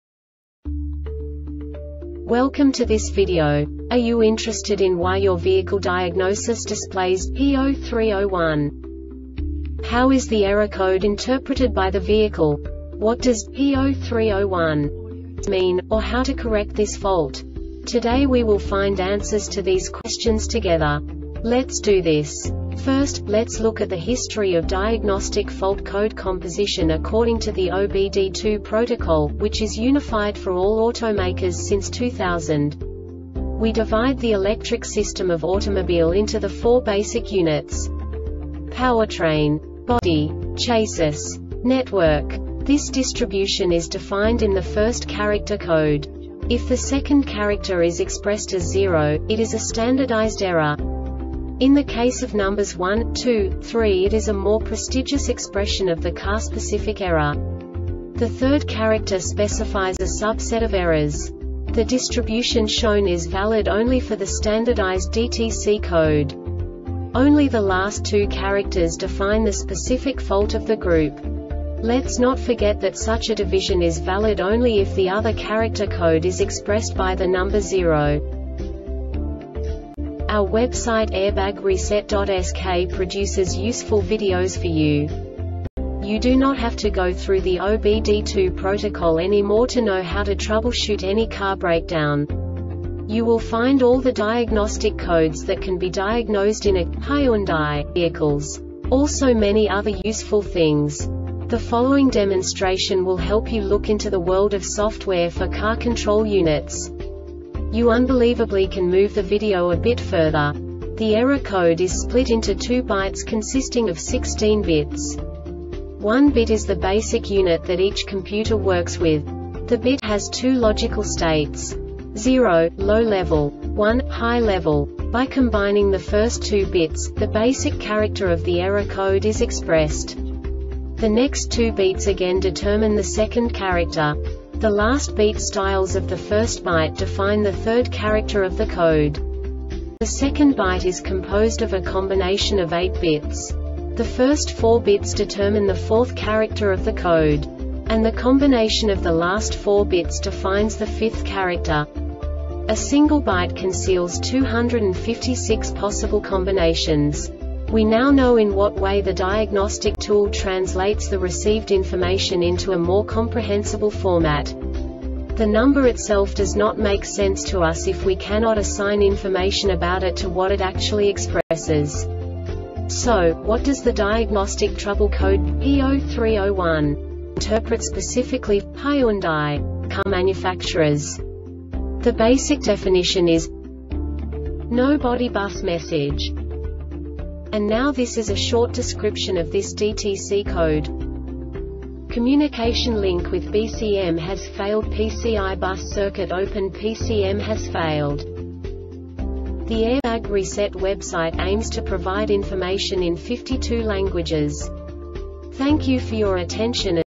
DTC Hyundai P0301 Short Explanation
Contents:
0:21 Basic DTC analysis according to OBD2 protocol standard.
1:48 Insight into programming
2:58 A diagnostically understandable description of the Diagnostic Trouble Code
3:05 Basic error definition

Make:
Hyundai
Code:
P0301
Definition:
No Body Bus Message
Cause: